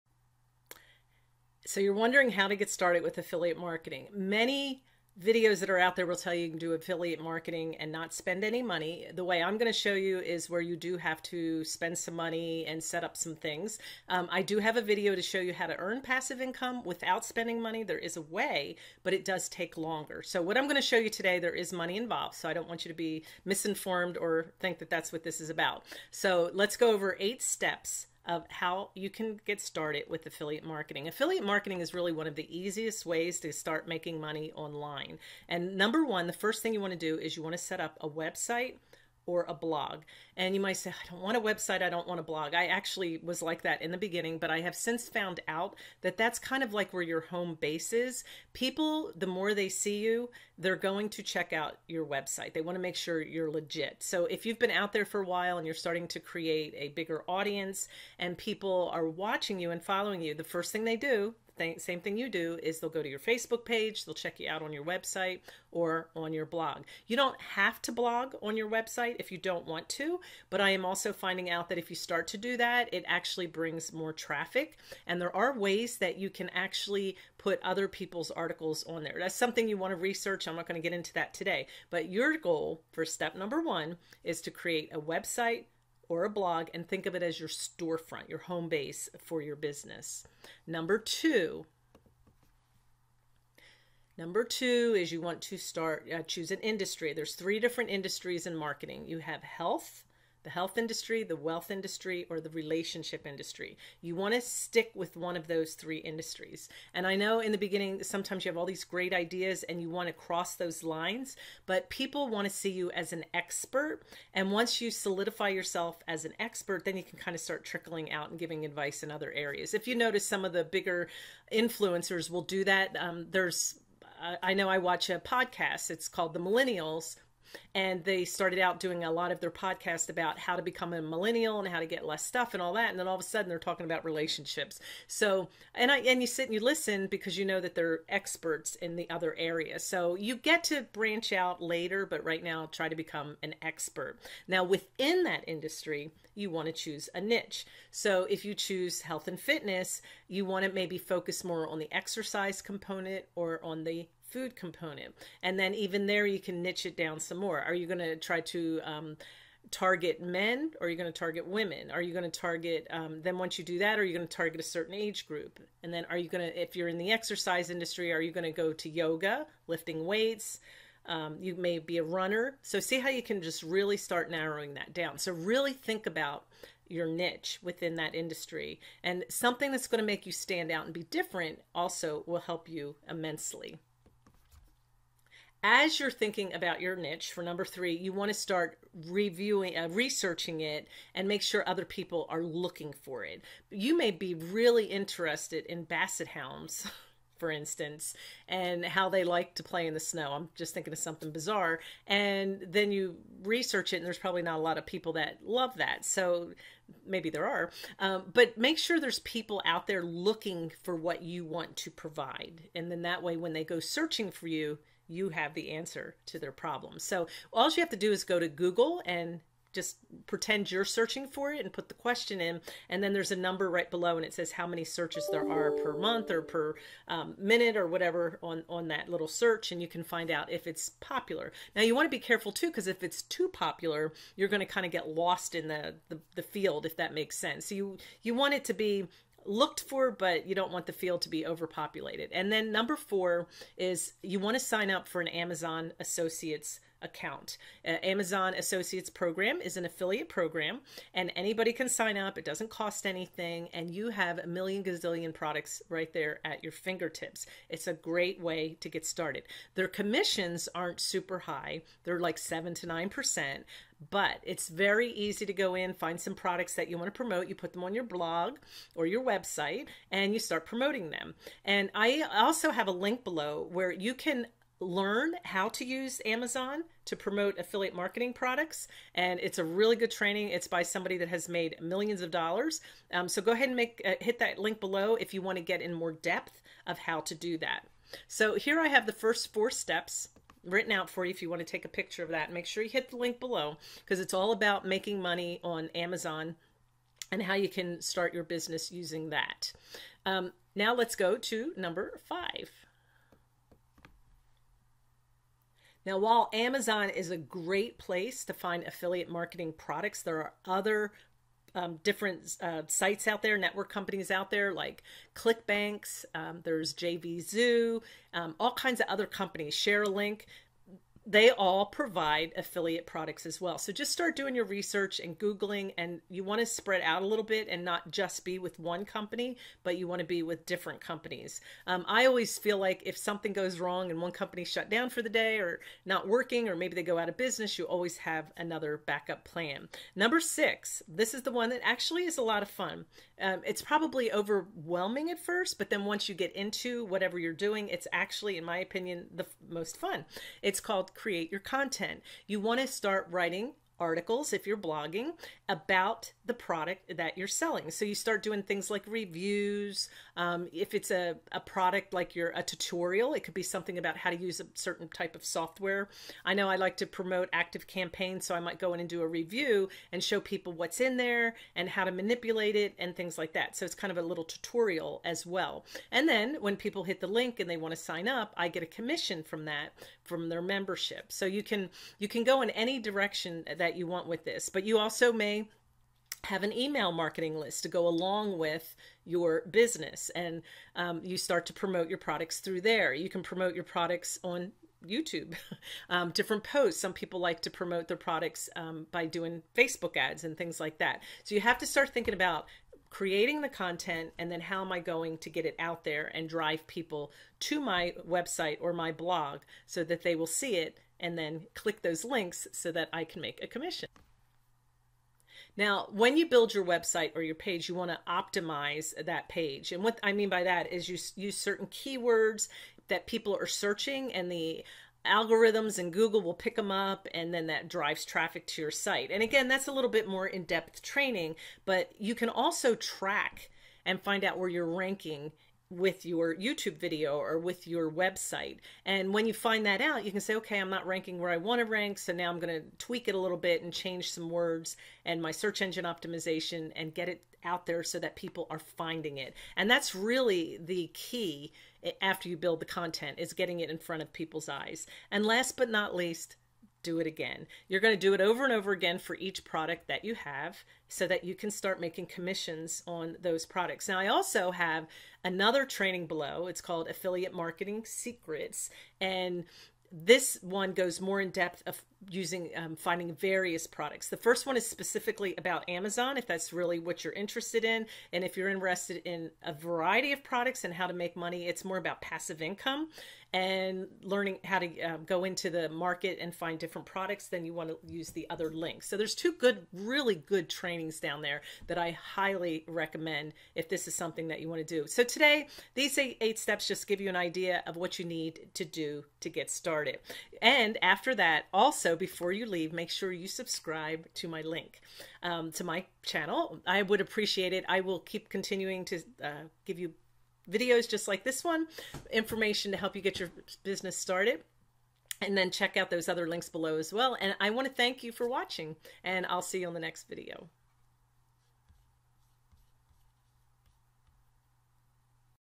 How to Get Started With Affiliate Marketing (8 Steps)
How to get started with affiliate marketing in 8 steps.  These 8 steps will help you to get started in your affiliate marketing business.  In this video I will review the steps you need to get started.  Once your business is set up you can start digging in and learning more skills to 10X your bottom line.